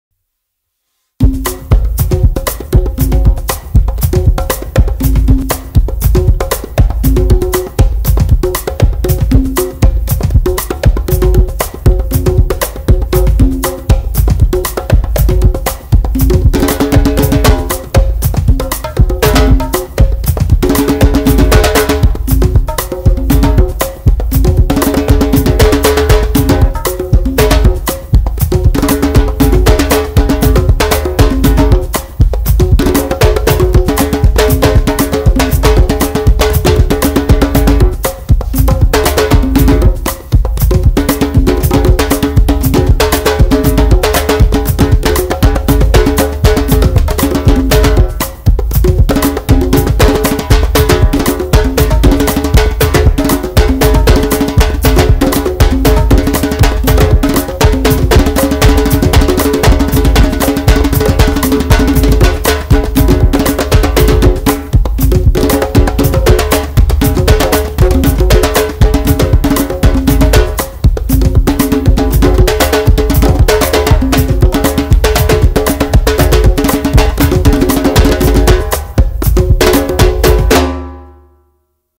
Pearl Percussion Travel conga's, bongo and Top Tuned Djembe
Playing a groove and solo on the Pearl Percussion Travel Conga's & Bongo and Top Tuned Djembe. The clip was made as a promo for Pearl Percussion
The drums are from the Dialects Loops Library

Headphones recommended!